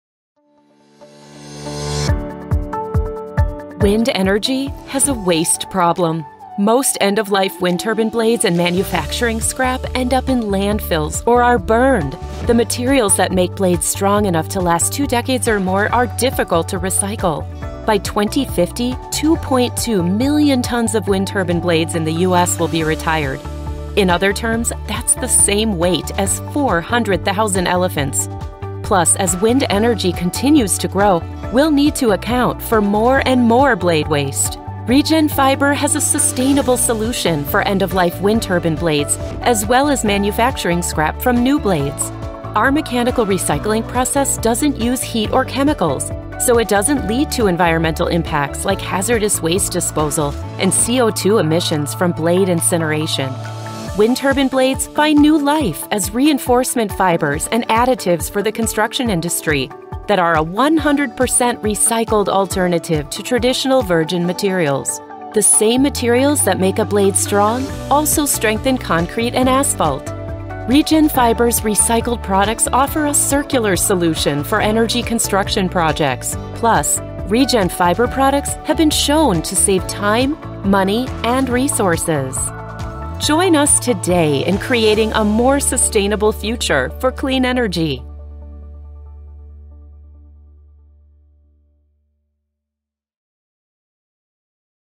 Wind Turbine Blade Recycling: A Sustainable End-of-Life Solution for Windmill Waste | REGEN Fiber
We turn waste into strength.

Most end-of-life wind turbine blades and manufacturing scrap are currently burned or buried — contributing to a growing environmental challenge known as the windmill graveyard. These durable materials are difficult to recycle, and by 2050, over 2.2 million tons of wind turbine blades will be retired in the U.S. alone.

At REGEN Fiber, we’ve developed a patent-pending, eco-friendly recycling process that gives these blades a second life. Our mechanical process doesn’t use heat or chemicals, which means no hazardous waste, no CO2 emissions, and no environmental fallout from incineration.

Instead, we mechanically break down the blades and convert them into reinforcement fibers and additives for the construction industry — a 100% recycled alternative to traditional virgin materials used in concrete, asphalt, and composites.

What makes REGEN Fiber different:

100% recycled construction fiber from wind turbine blade material
No incineration, no toxic byproducts, no landfill waste
Stronger, more sustainable concrete and asphalt
Proven to save time, money, and resources
Supports infrastructure projects in Texas and other wind-heavy states

As wind energy continues to grow, we must find a better way to manage its waste. Windmill blade disposal doesn't have to mean landfills or incinerators. With REGEN Fiber, we can build a cleaner, stronger future — powered by recycled wind turbine blades.